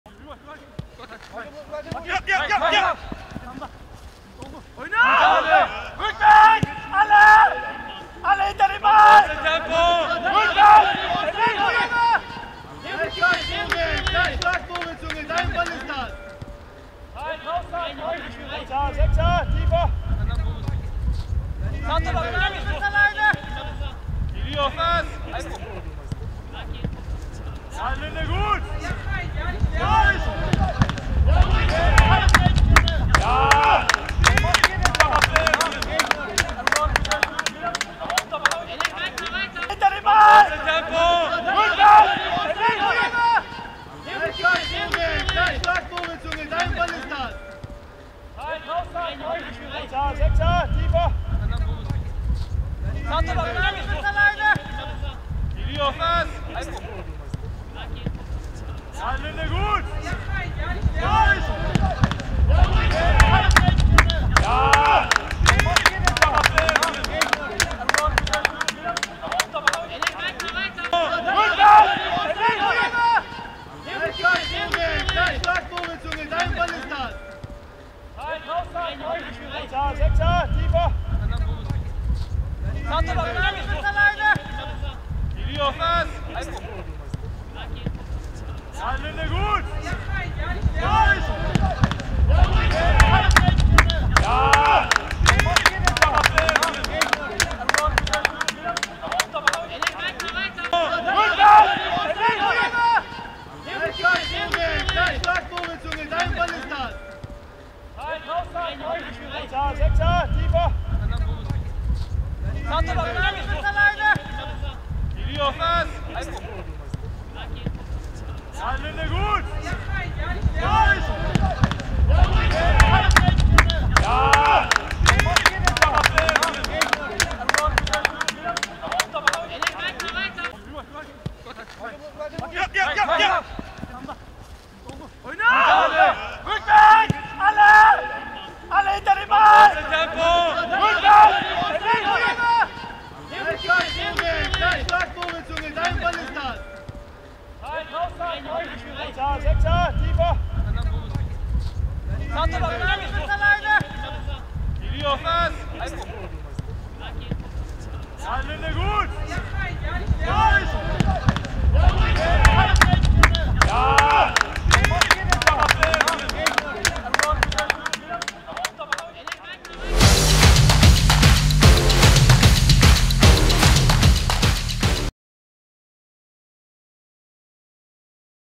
Tor (TSG) Vahdet Salzgitter TSG Bad Harzburg 4zu1
KSV Vadet Salzgitter vs. TSG Bad Harzburg  zum 4:1 (Tor TSG)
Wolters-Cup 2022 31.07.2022